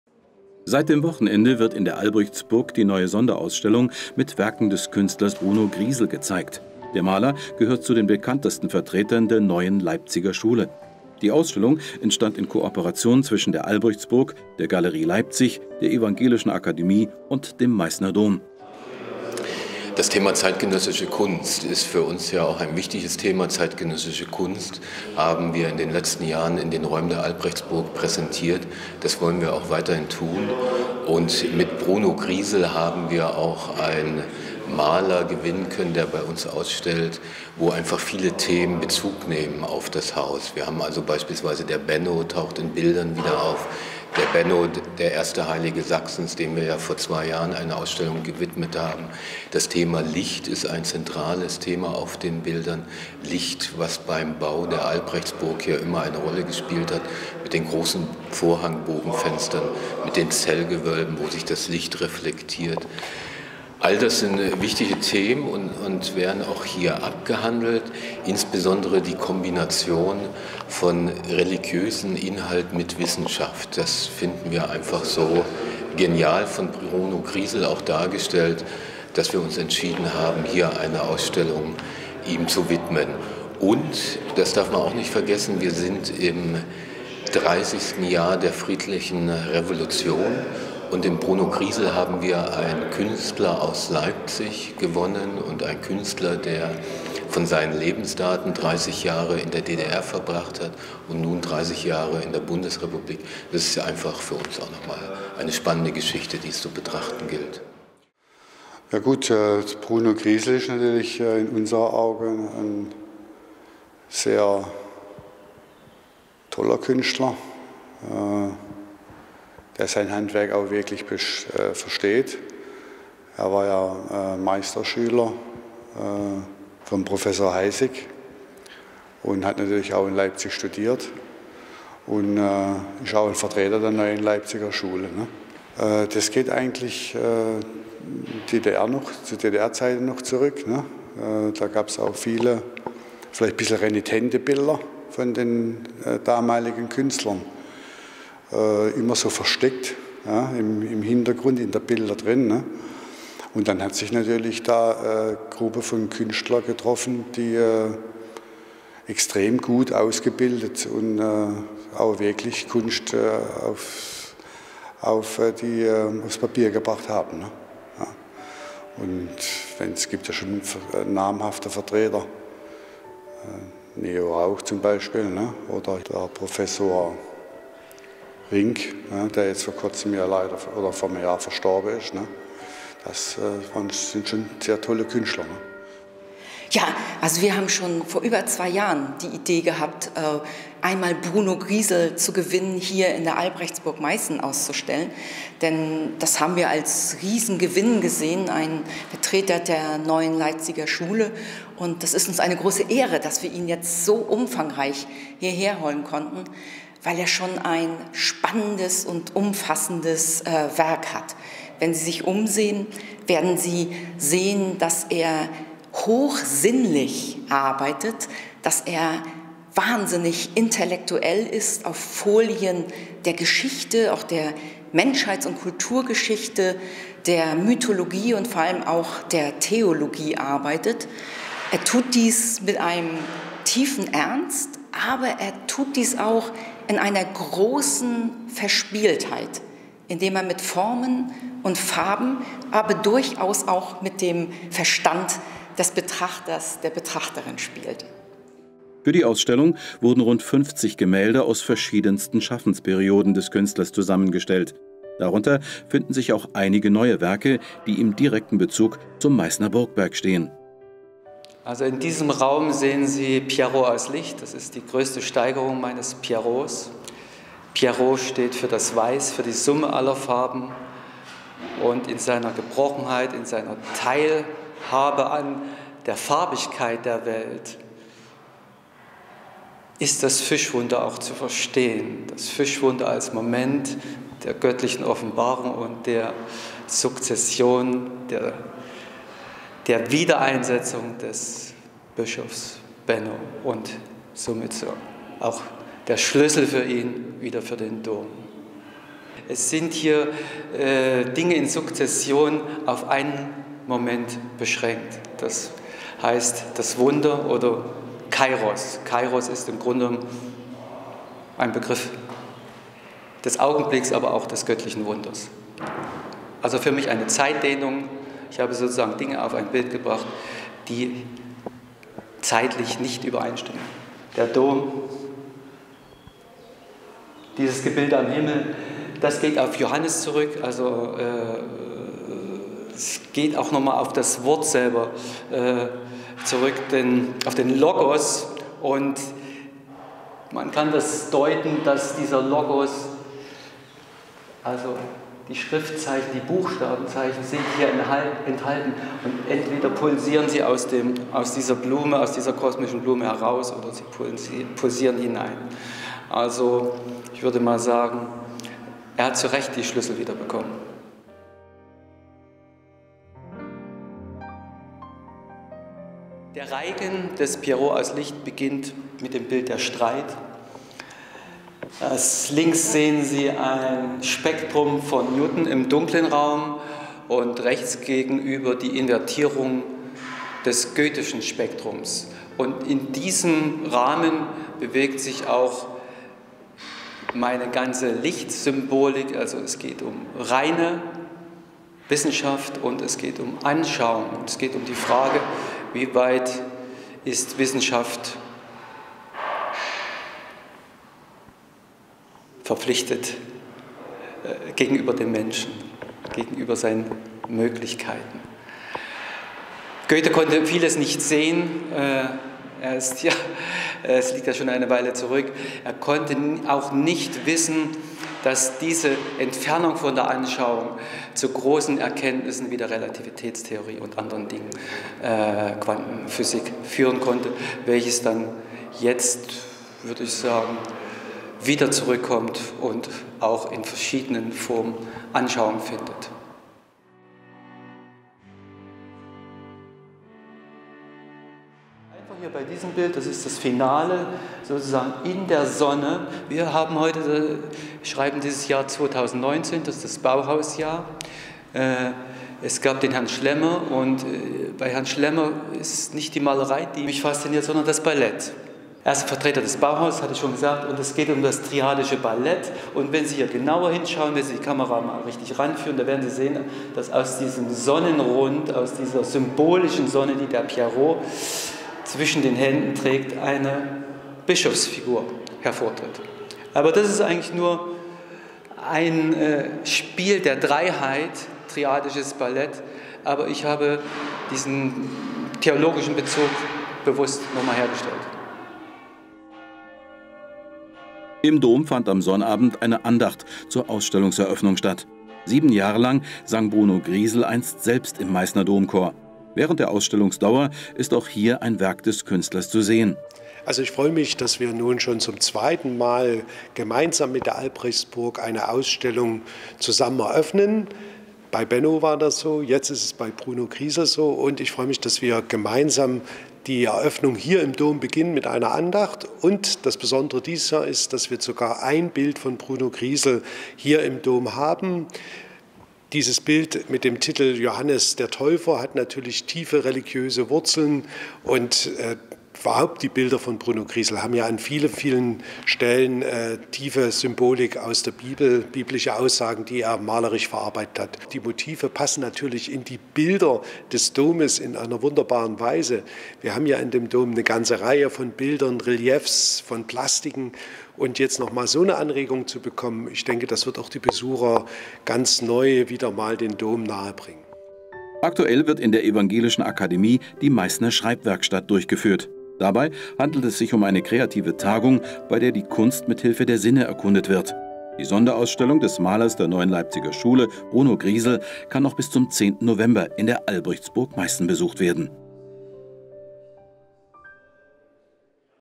Neue Leipziger Schule mit Bruno Griesel in der Albrechtsburg Meissen
Anlässlich der Sonderausstellung, bei der vom 13.07. bis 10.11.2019  Werke des Künstlers Bruno Griesel ausgestellt werden, wurde ein Beitrag durch Meissen TV erstellt.